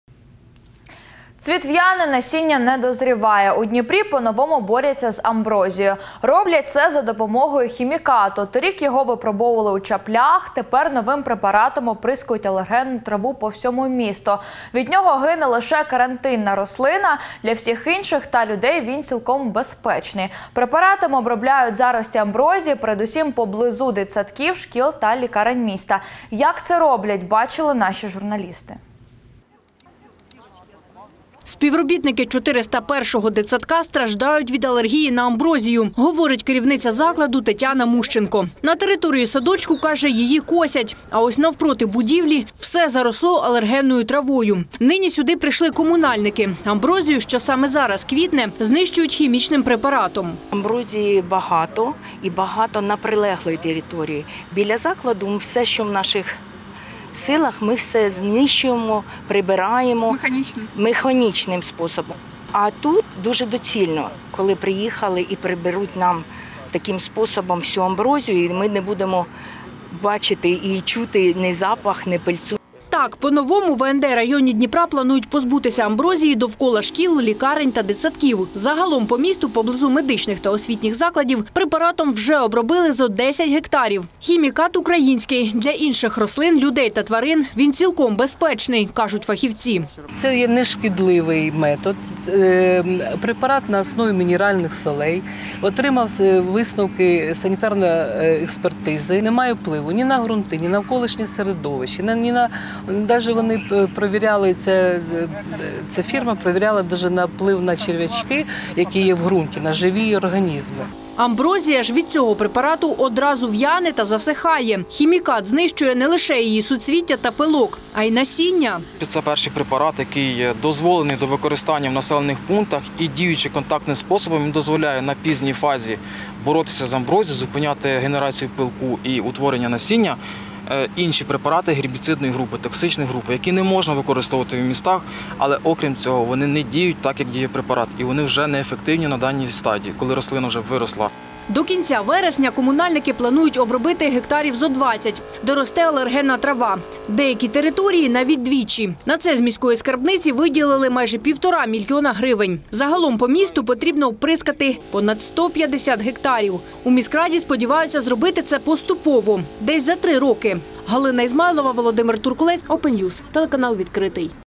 Цвіт в’яне, насіння не дозріває: у Дніпрі по-новому борються з амброзією
Цвіт в’яне, насіння не дозріває. У Дніпрі по новому борються з амброзією Роблять це за допомогою хімікату. Торік його випробували у Чаплях, тепер  новим препаратом обприскують алергенну траву по всьому місту.  Від нього гине лише карантинна рослина. Для усіх інших та людей він цілком безпечний. Препаратом обробляють зарості амброзії передусім поблизу дитсадків, шкіл та лікарень міста.